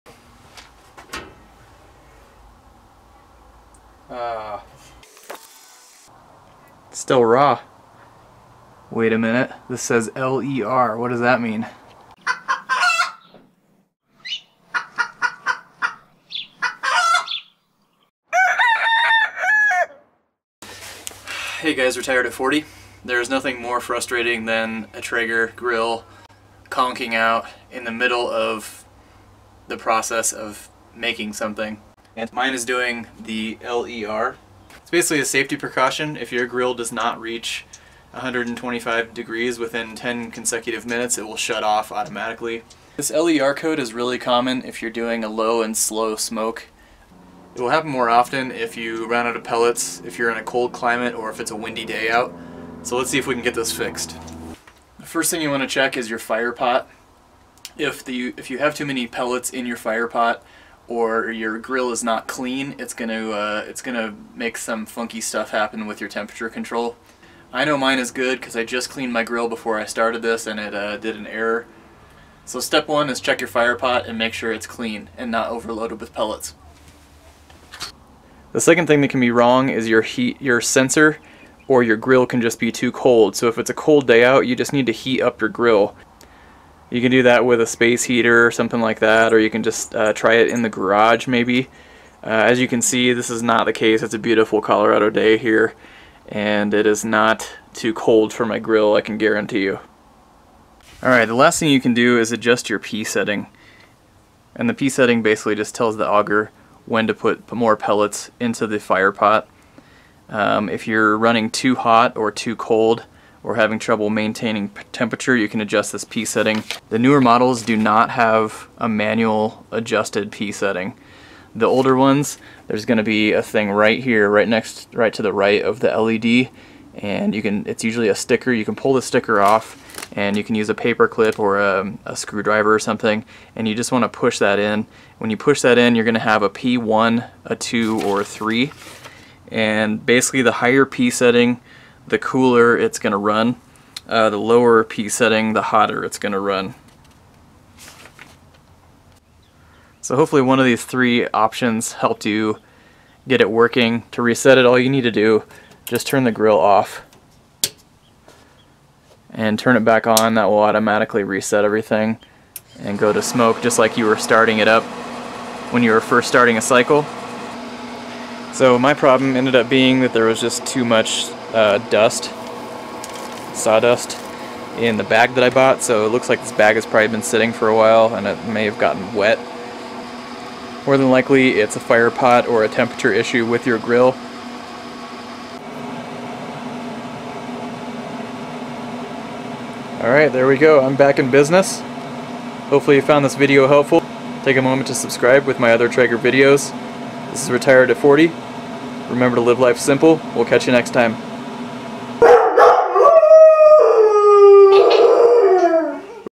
TRAEGER TROUBLESHOOTING👉How to fix "LEr" Error Code👈
There i was minding my own business and my Traeger shut off and said "LEr" on the screen.  Here's what the cause is and how to fix it.
See my other Traeger issues and troubleshooting videos on how to solve error codes and heating issues.  HEr is also a common error code.  See the video below.